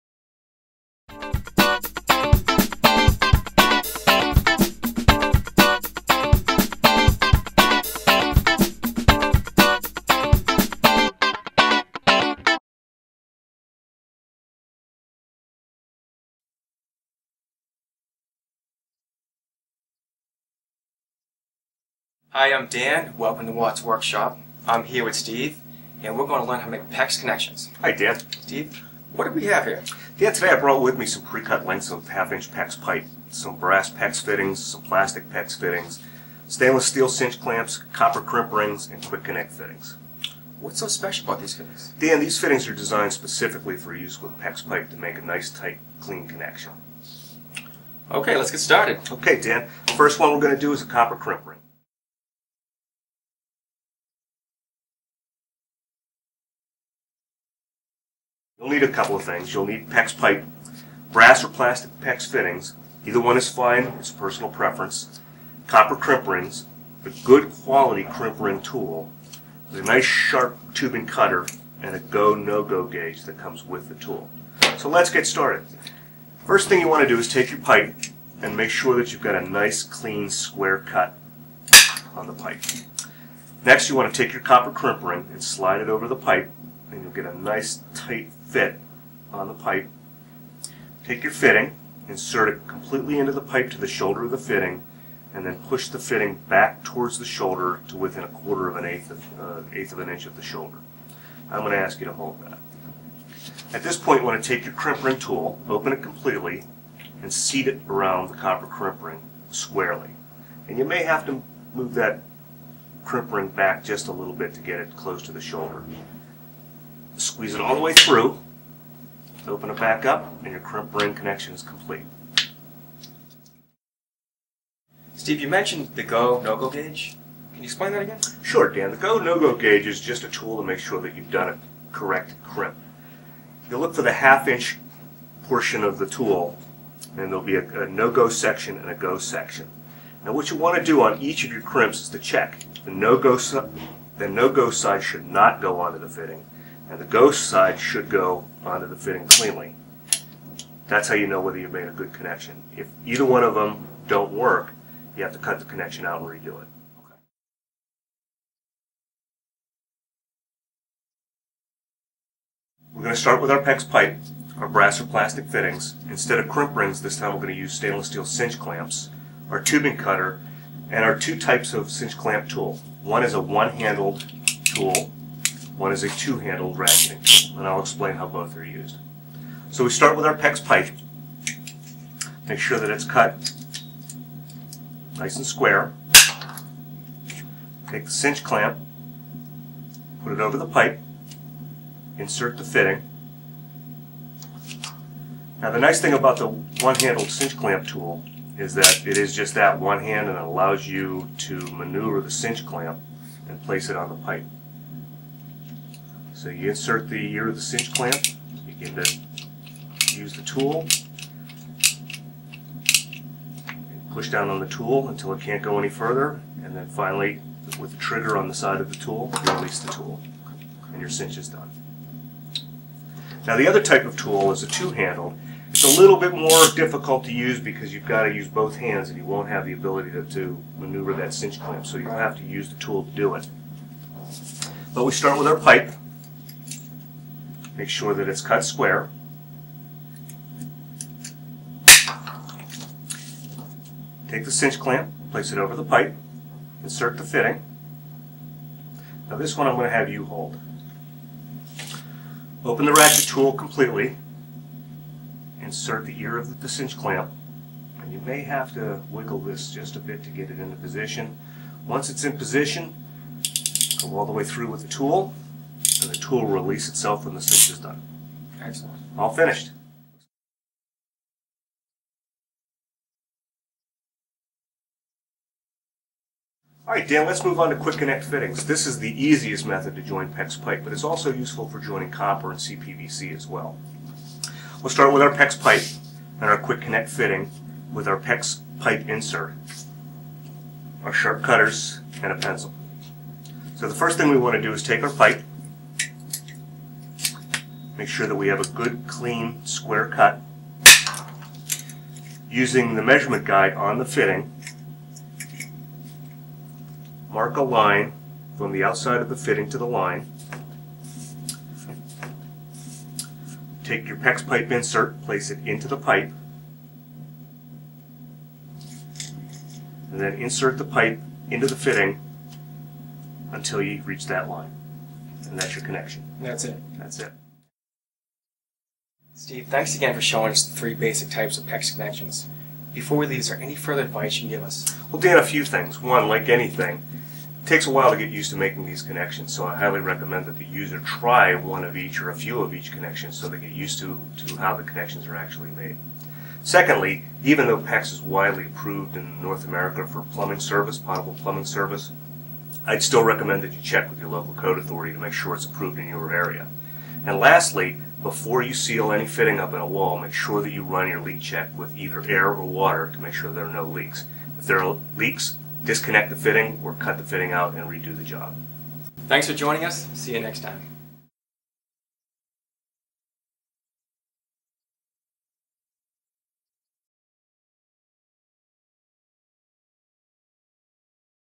How to Connect PEX Pipe with Watts WorkShop™
In the Workshop, Dan and Steve walk through three unique methods for the installation of PEX tubing including copper crimpring connections, stainless steel cinchclamps and quick-connect fittings.